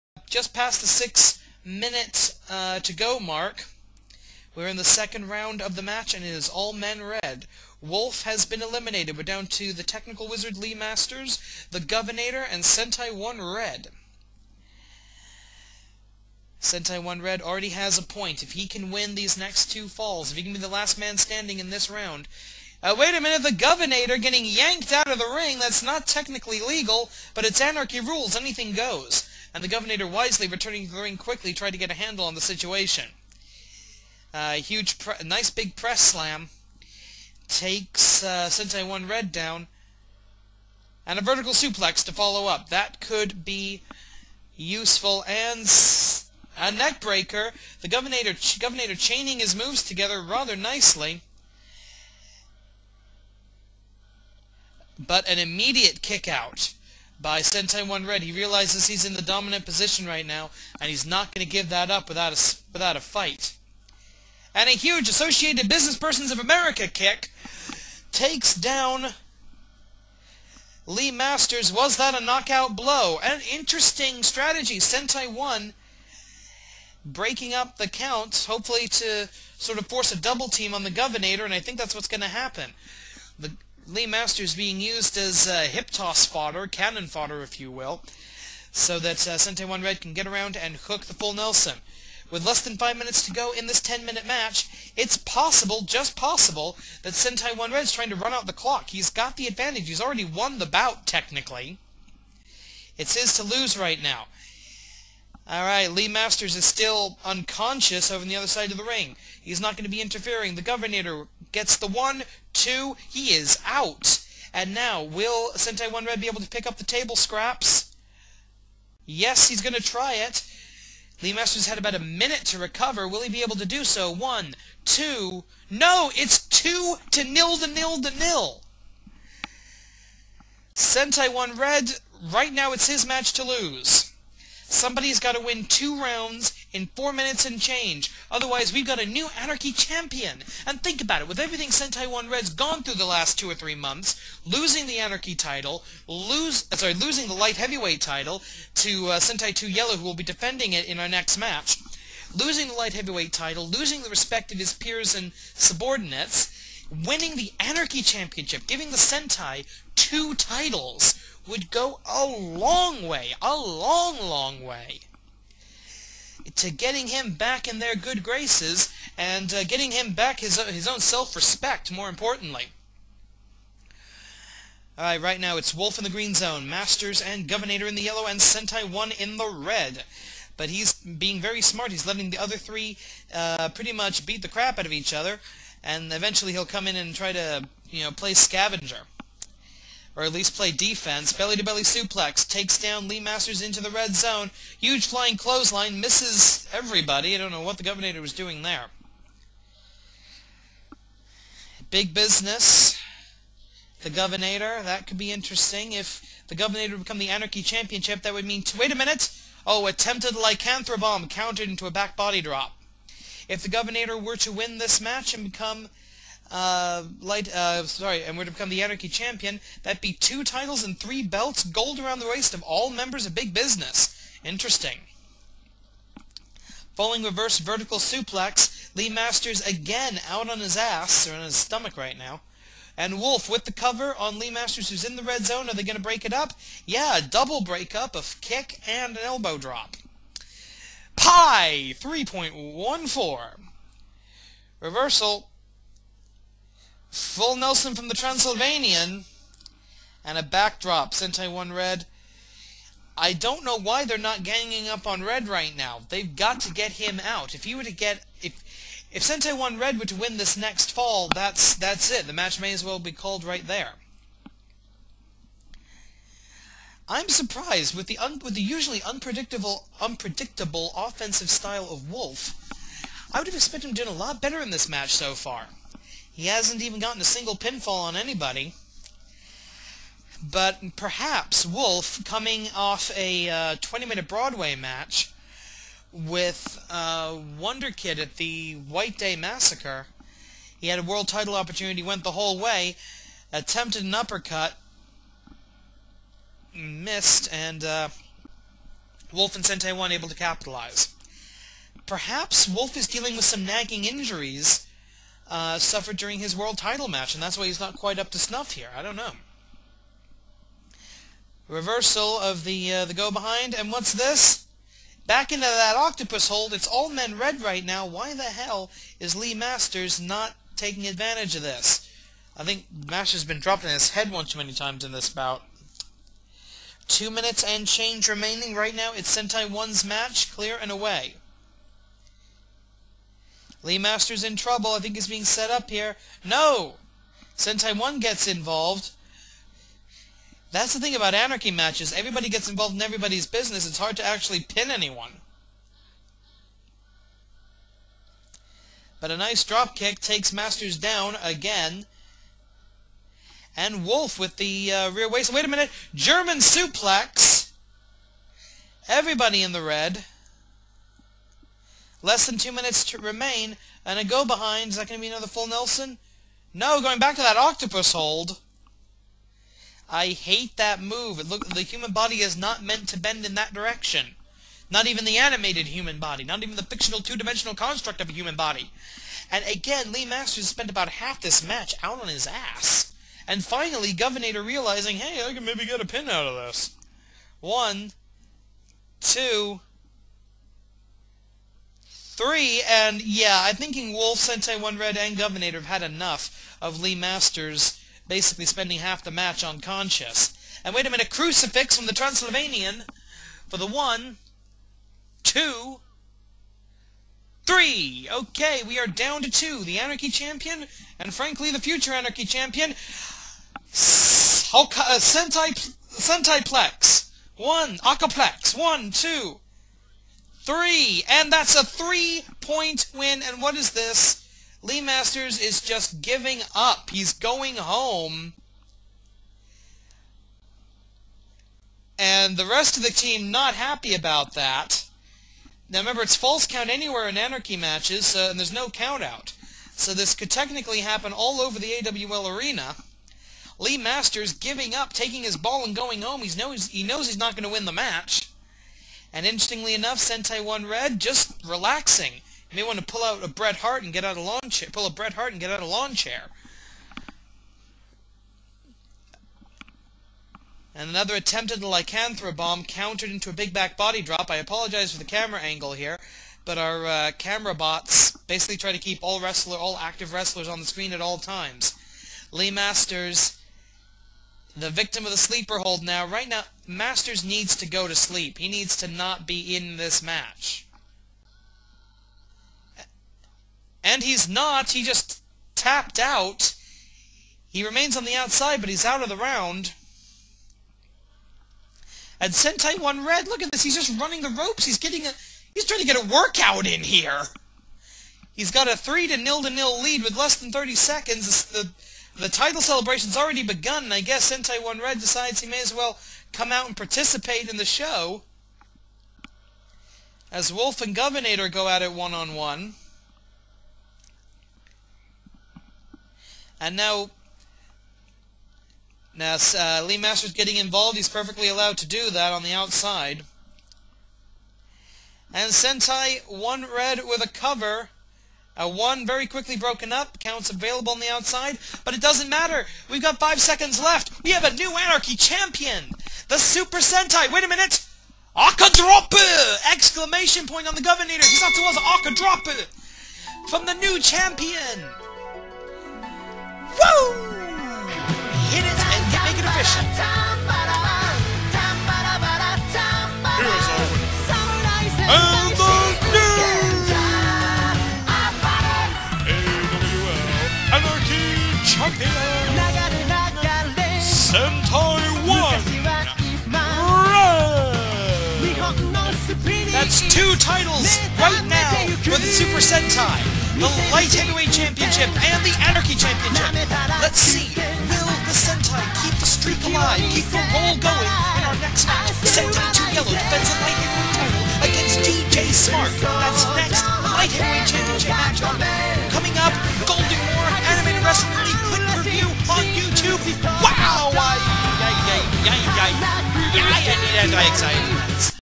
AWL Click Per View Golden War GW Match 5 pt 2
ANARCHY Match!

The Anarchy Title Match concludes.  Which faction will take home the  AWL Anarchy Championship?  Or will Wolf take the title back to the pack?